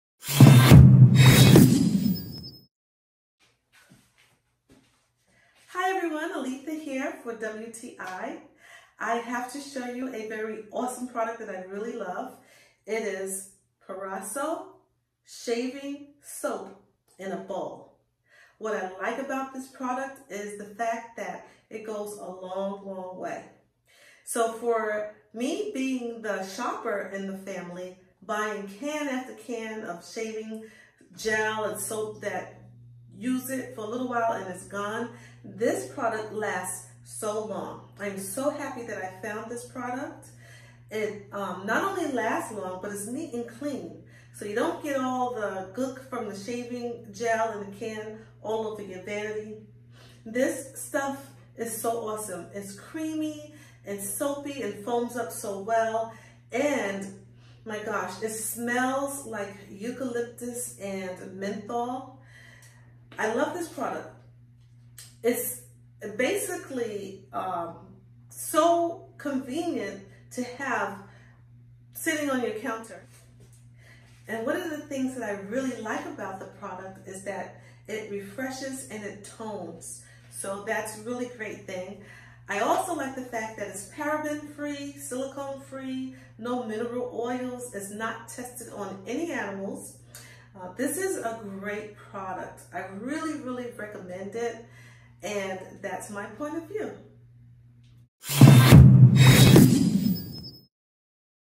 Our Point of View on Proraso Shaving Soap In A Bowl
(Commissionable Links)

about this PRODUCT:Proraso Shaving Soap In A Bowl - Refresh, 5.2 Oz